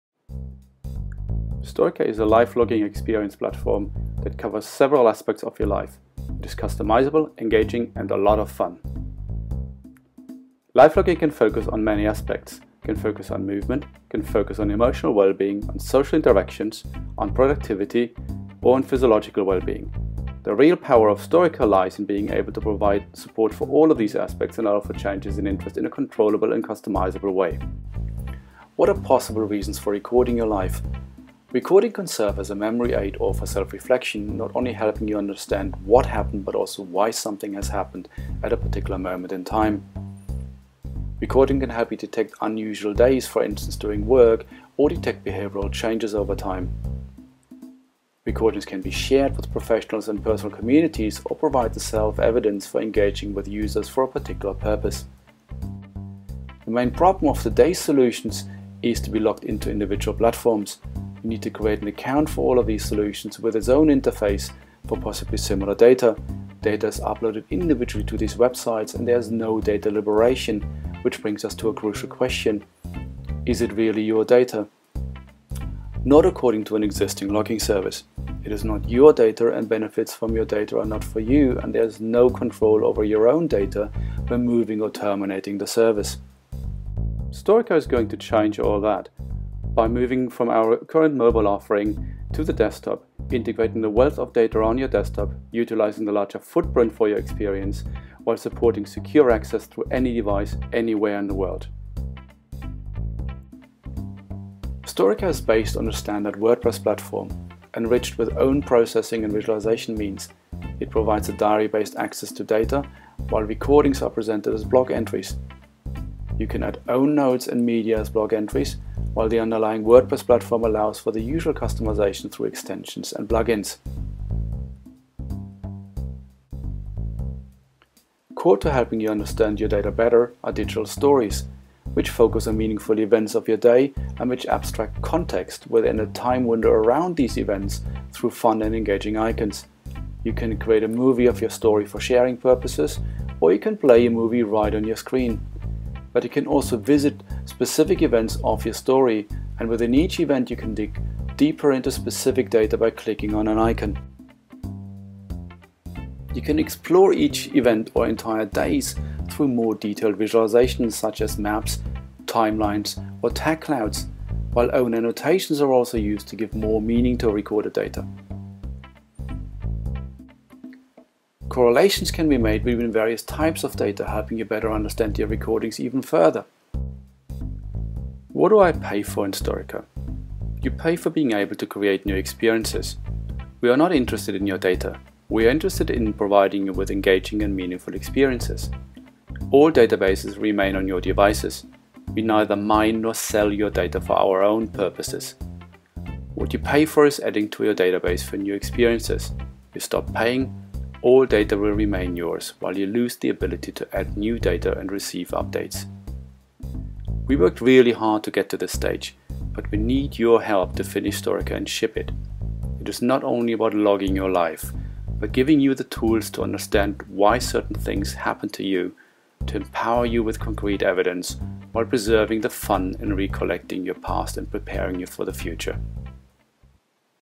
Kickstarter Project Storica
This video brings to you the Storica lifelogging platform Kickstarter project, which will launch soon!

Check out our kickstarter project website (search for "Storica" in for the various pledges and how you can support making Storica a reality!